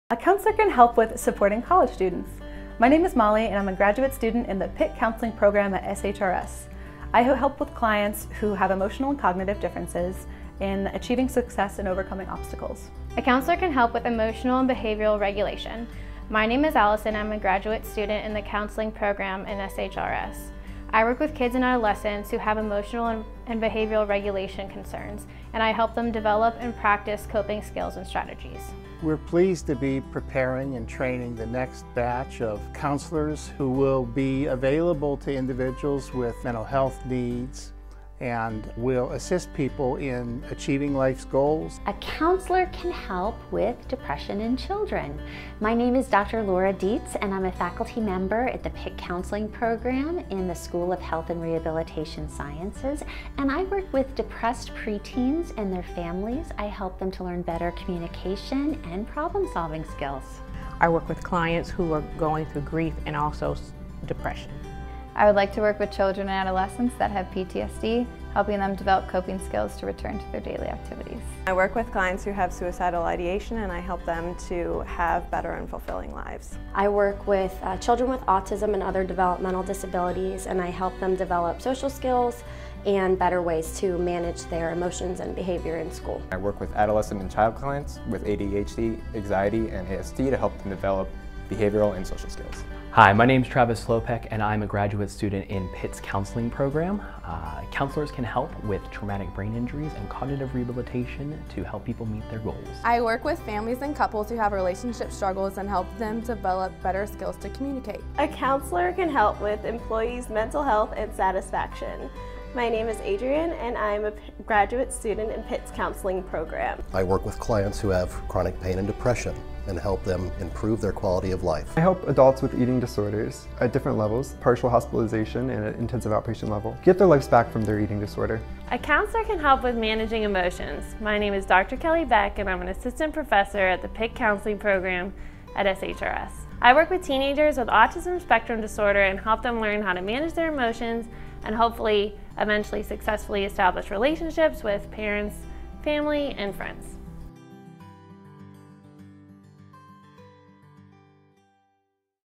Counselors Help...
Here at Pitt SHRS, we train the next generation of counselors to help people overcome a wide array of obstacles to lead fuller, happier, more fulfilling lives. Learn more about the Counseling program at shrs.pitt.edu.